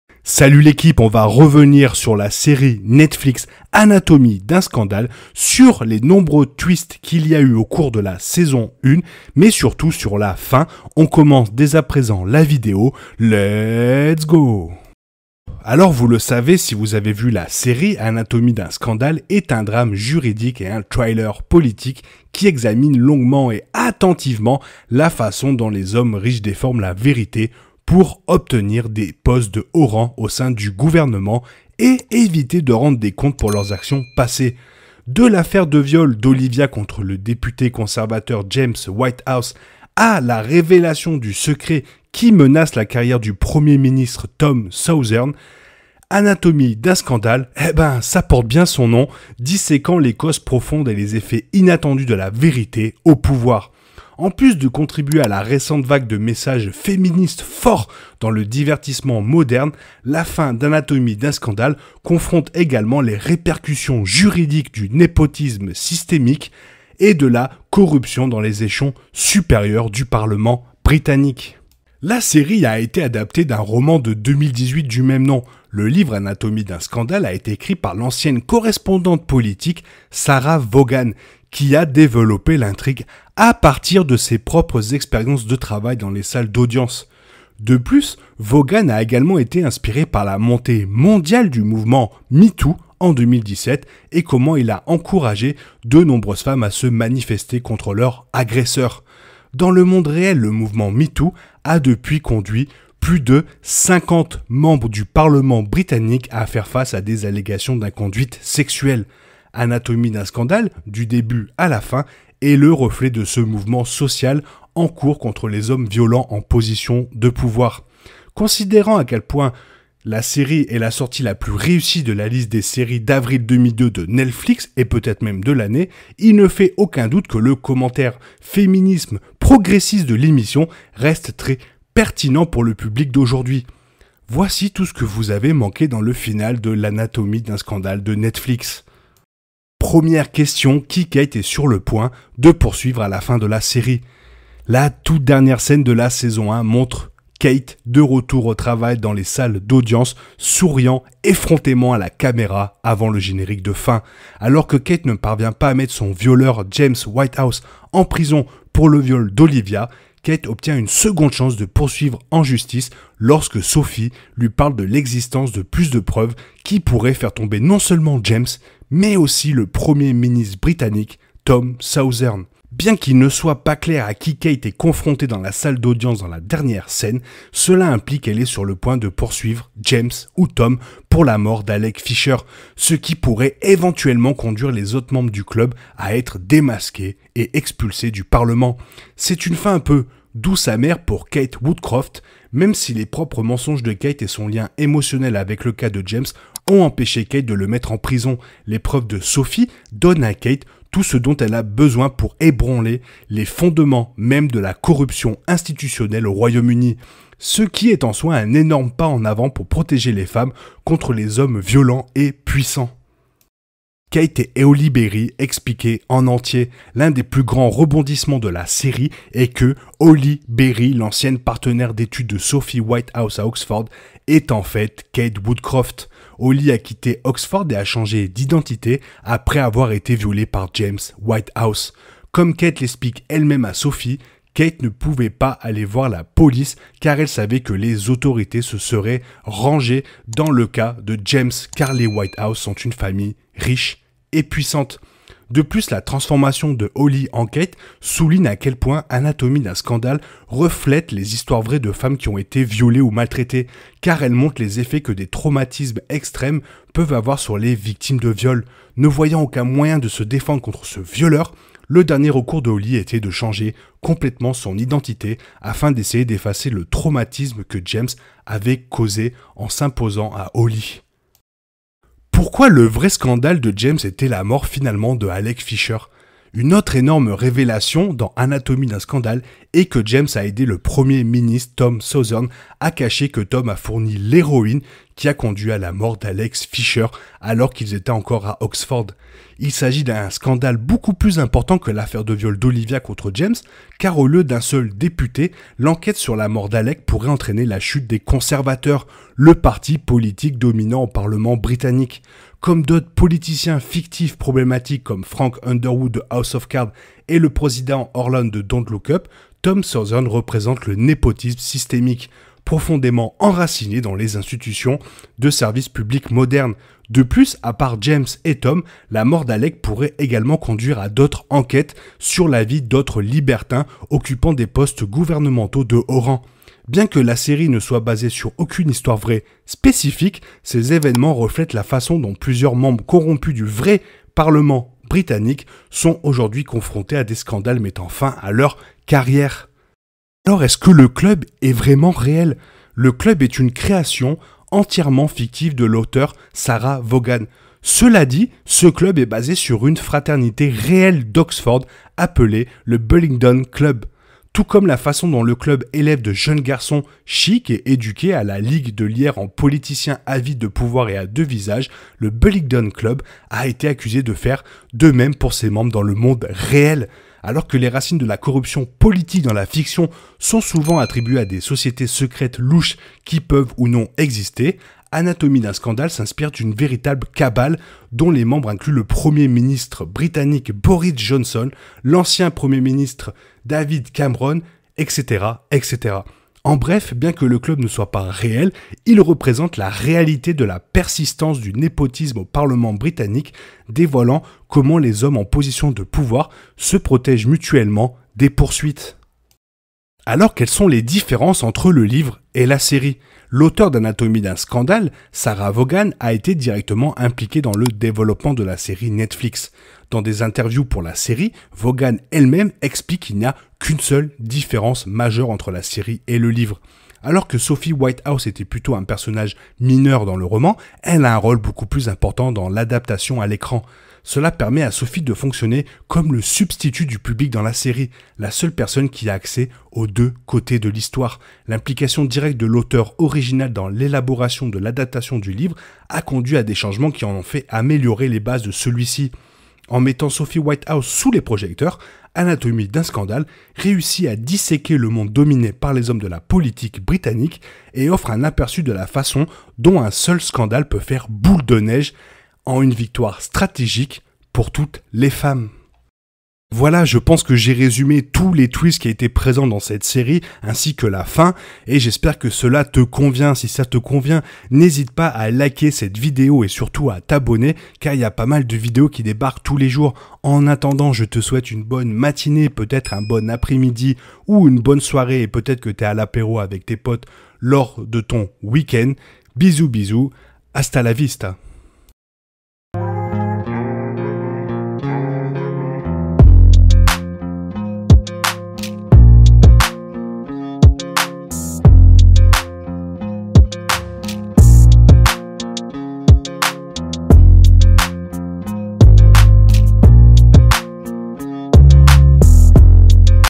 ANATOMIE D'UN SCANDALE FIN EXPLIQUÉE !
Voici toutes les explications sur les Twists et la fin de cette série.

Sur TWITTER, INSTAGRAM et TIK TOK : @therapiegeek